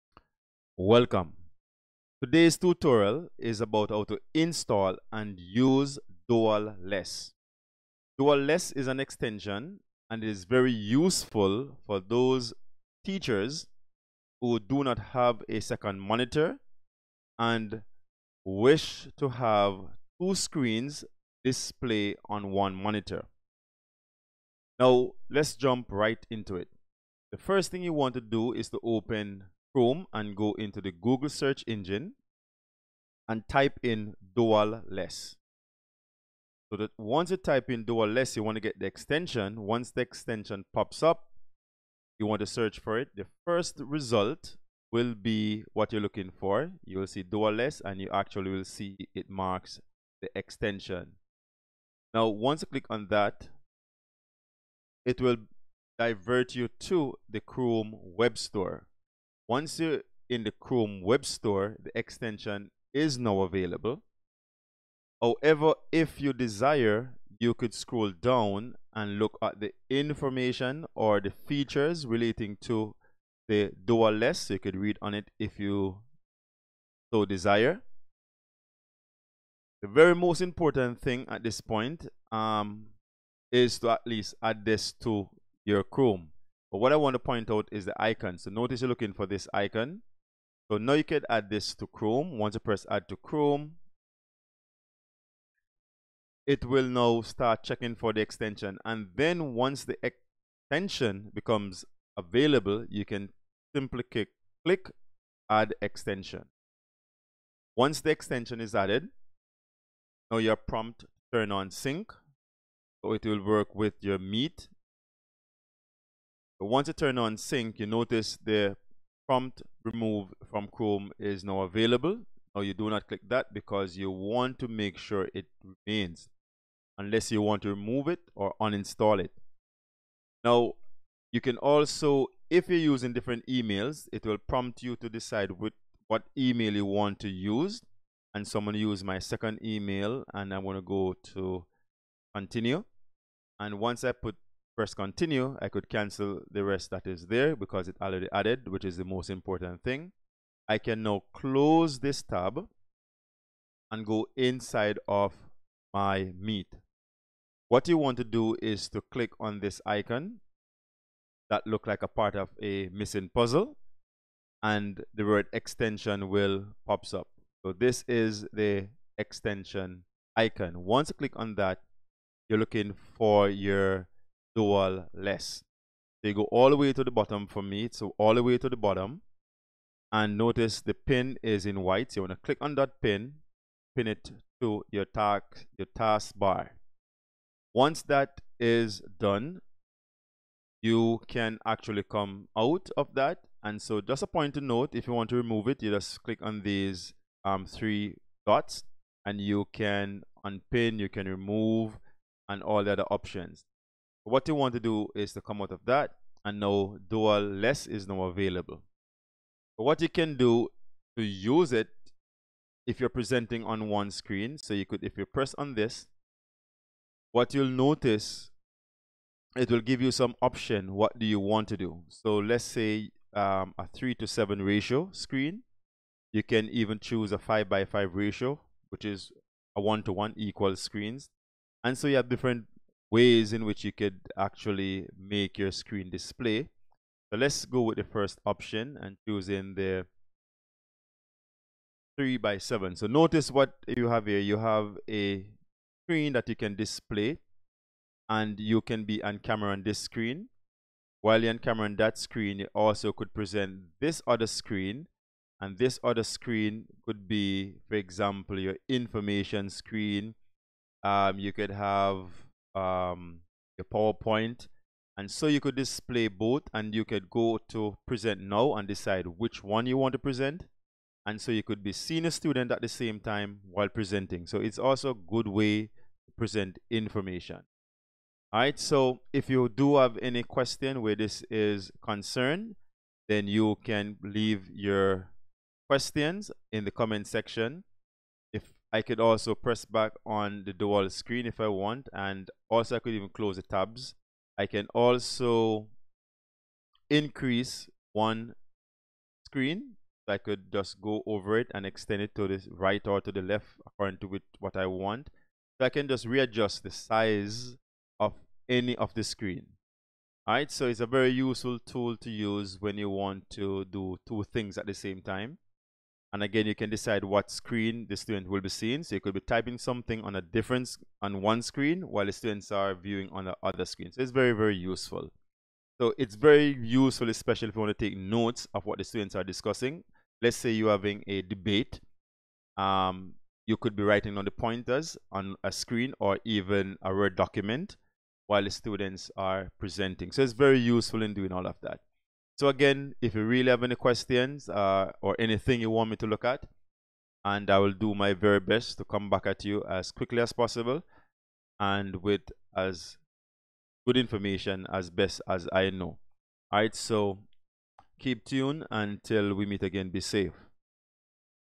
Using Dualles (screen split screen) in google meet. Installing and using the extension for Dualless.
This tutorial explains and demonstrates how to install and use the dualless extension to split your screen in google meet.